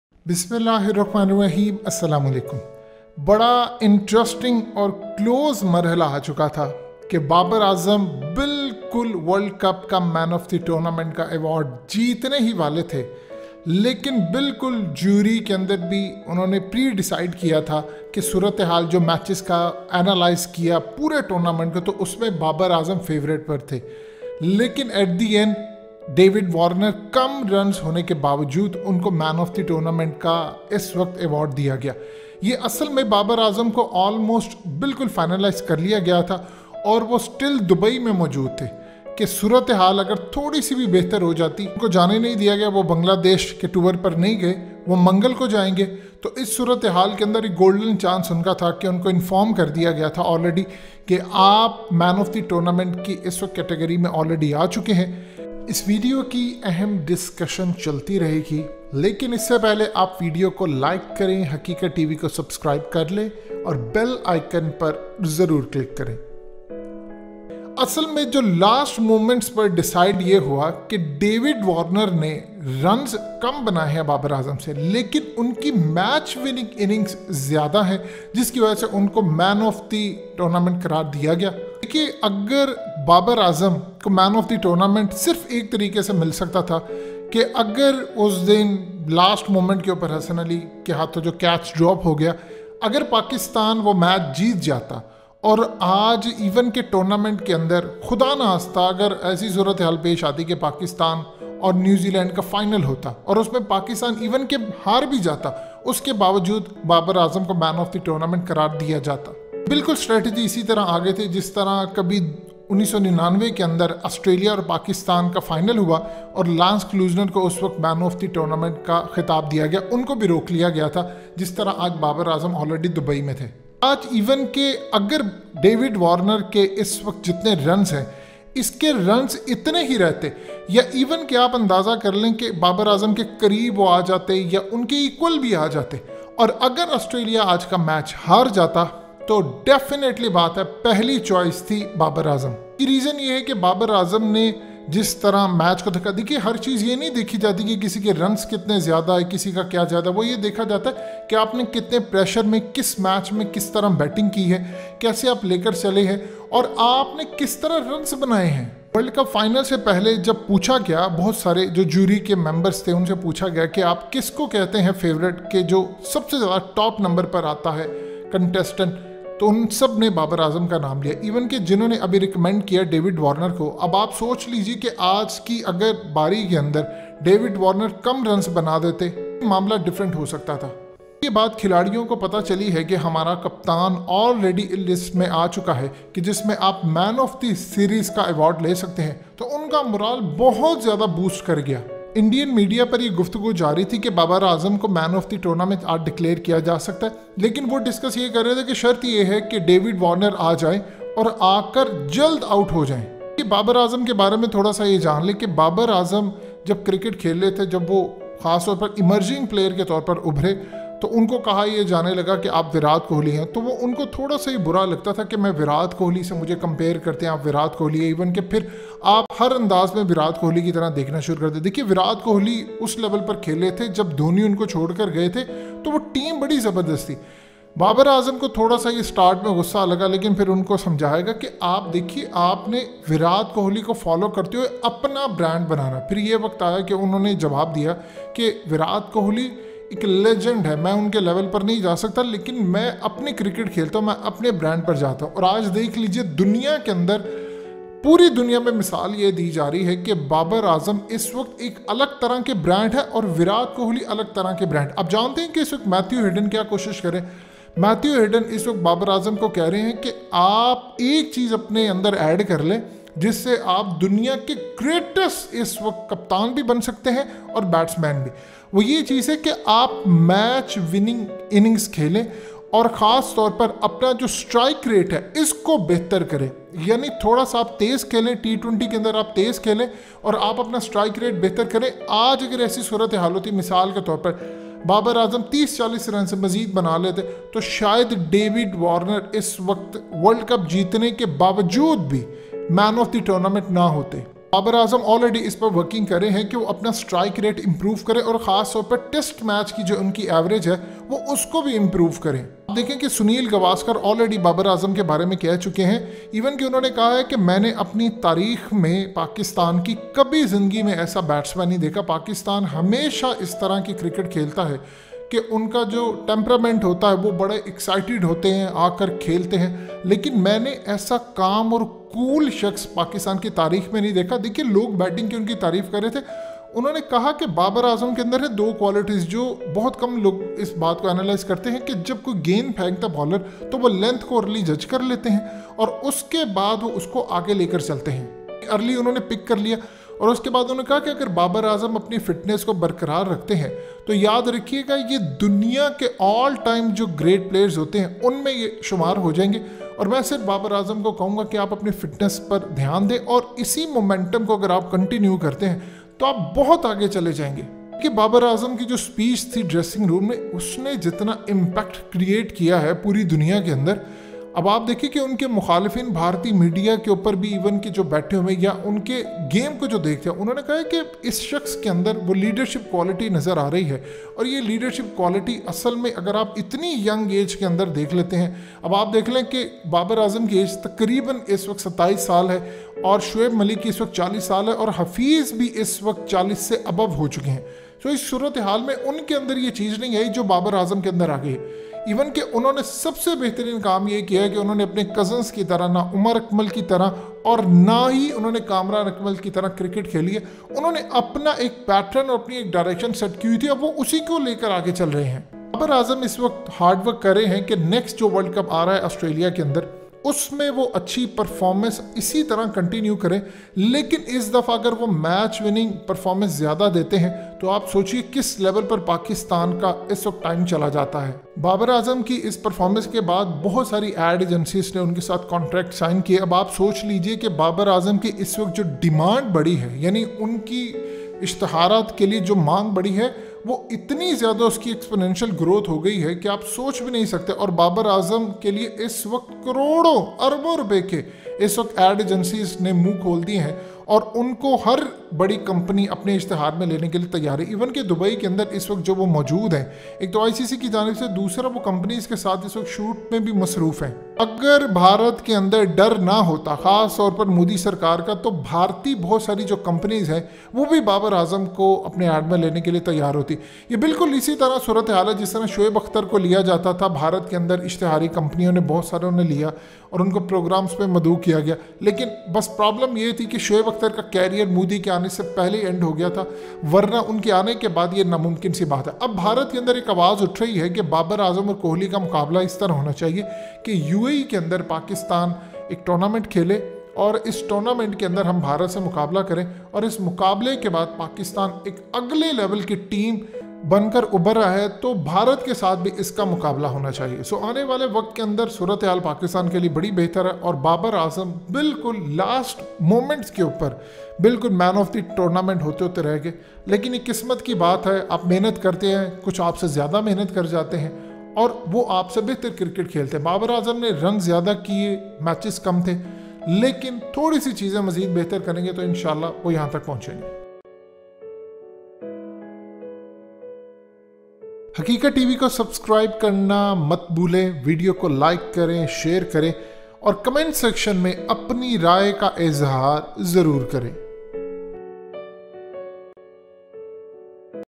Why David Warner was awarded Man of the Tournament instead of Babar Azam?
Cricket fans all over the world including Pakistan were thinking that this time Babar Azam would be given the man of the tournament award but he was replaced by David Warner because of his good batting which made many people unhappy.